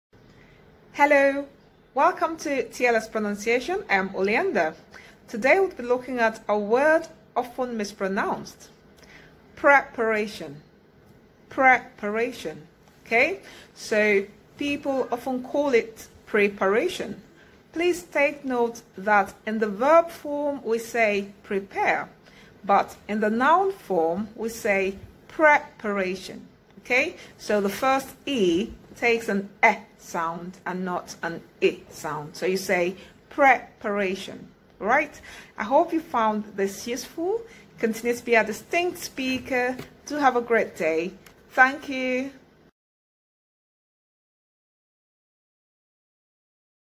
How to pronounce 'Preparation'
Do not pronounce the 'pre' in preparation the way you would say 'prepare'. Say it like the 'pre' in 'pressure'